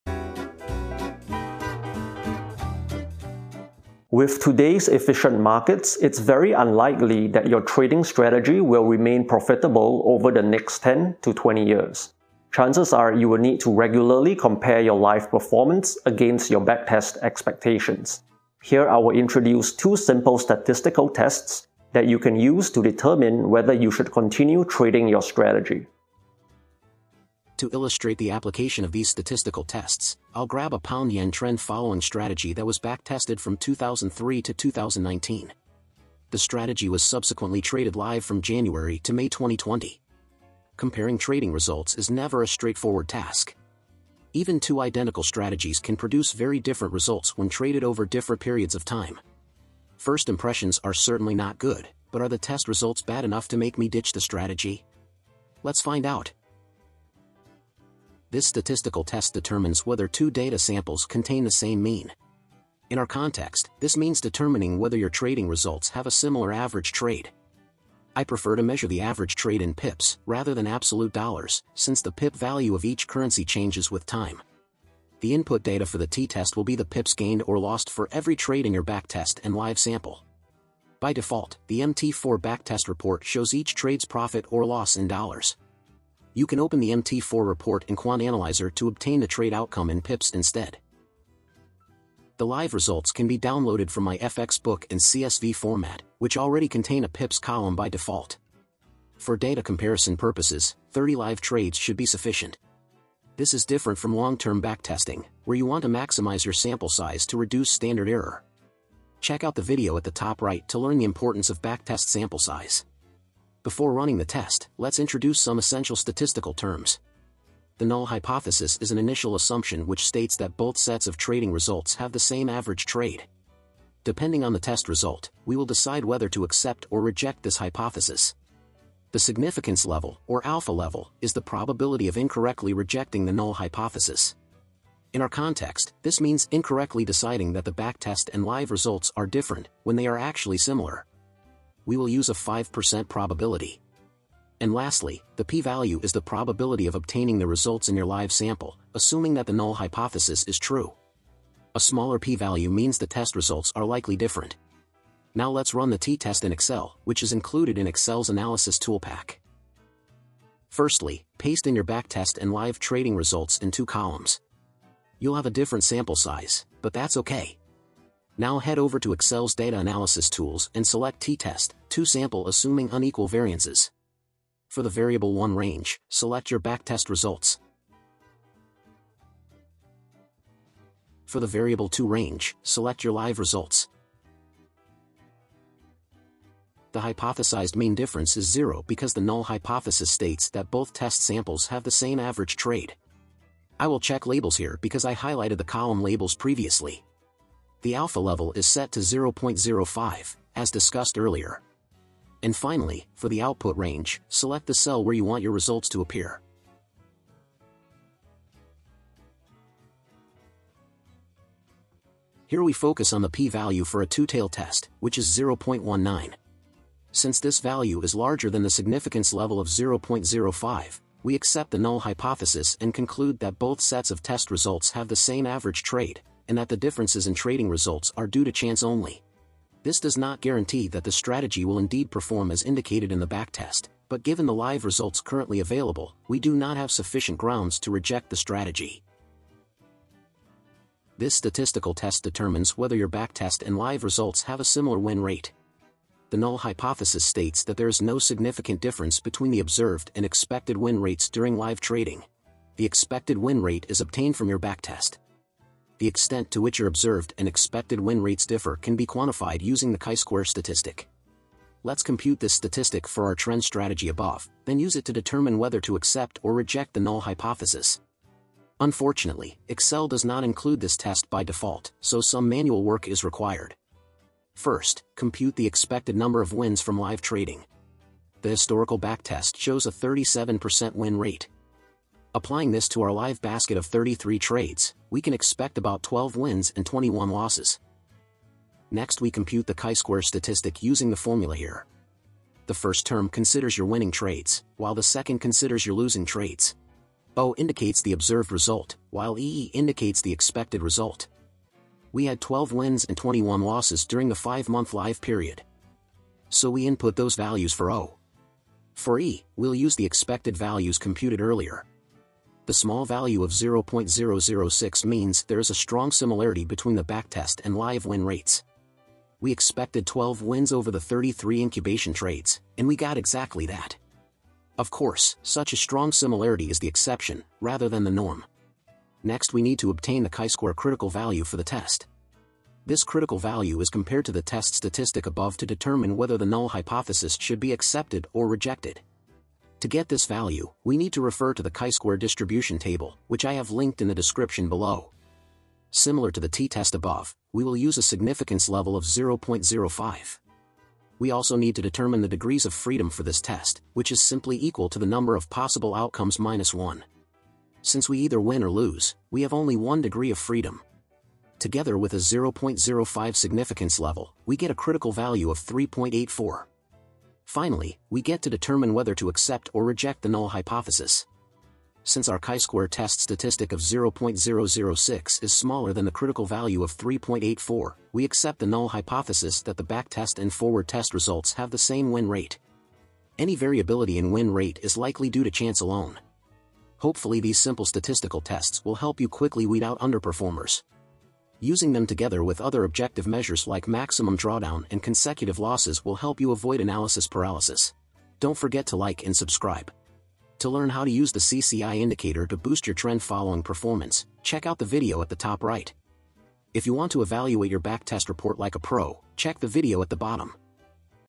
Using Statistics For Trading Performance Evaluation
Is your trading strategy’s LIVE performance in line with your backtest results? Find out by applying these simple statistical tests!

✅ Copy my trades on Darwinex!

⏱️Timestamps:
0:00 Introduction
0:27 A Tale Of Two Strategies
0:57 Student's t-test
4:00 Chi-square Test